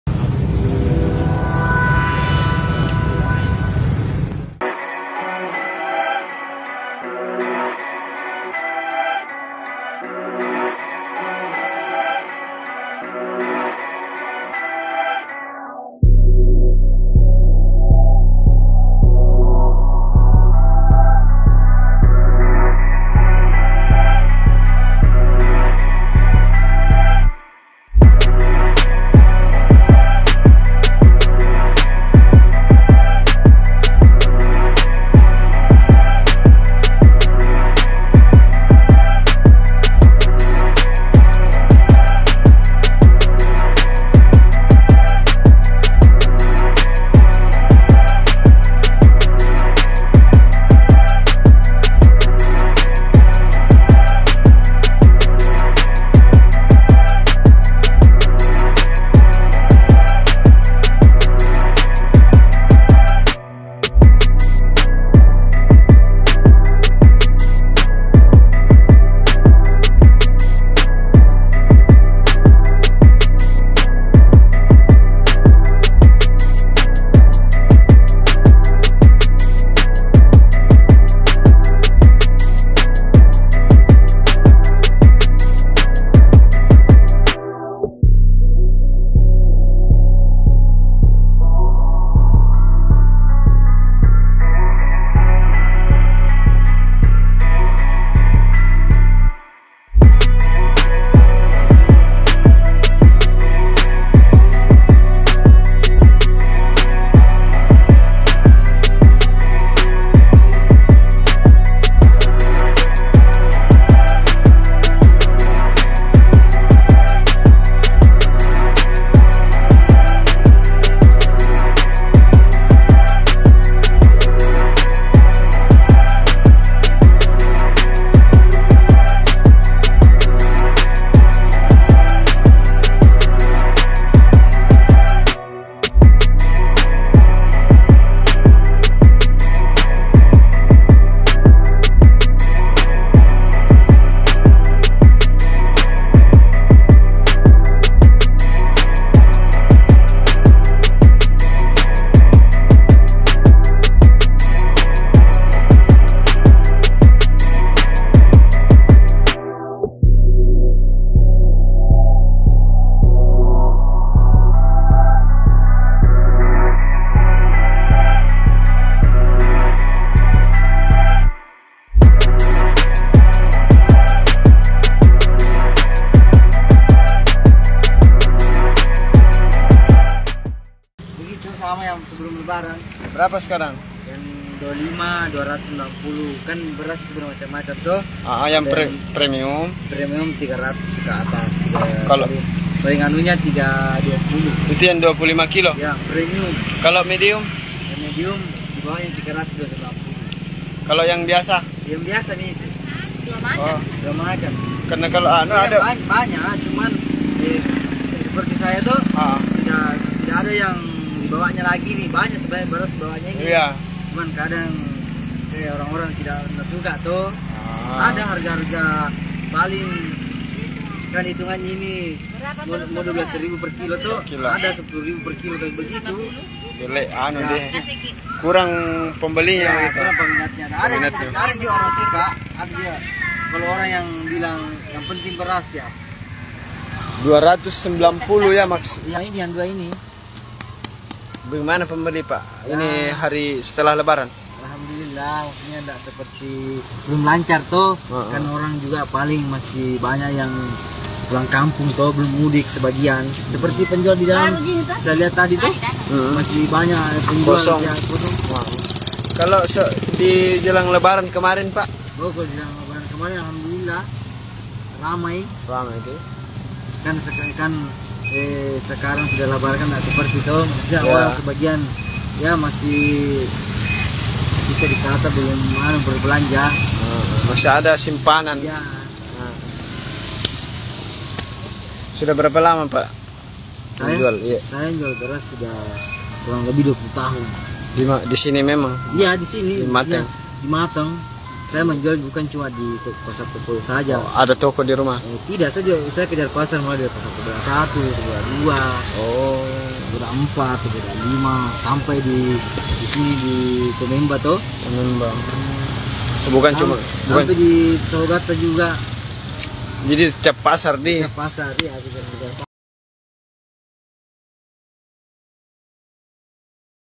Aktivitas di Pasar Topoyo Mamuju Tengah Normal Sepekan Usai Lebaran
TRIBUN-SULBAR.COM, MAMUJU TENGAH - Sepekan usai lebaran aktivitas di Pasar Topoyo, Mamuju Tengah kembali normal.

Berdasarkan pantauan, Jumat (28/4/2023) aktivitas jual beli normal seperti pada hari biasa sebelumnya.

Meski terlihat beberap toko dan lapak pedagang masih tertutup, namun pengunjung pasar terlihat ramai. Hari ini merupakan hari pertama pasar Topoyo beroperasi pasca lebaran Idul Fitri 1444 Hijriah.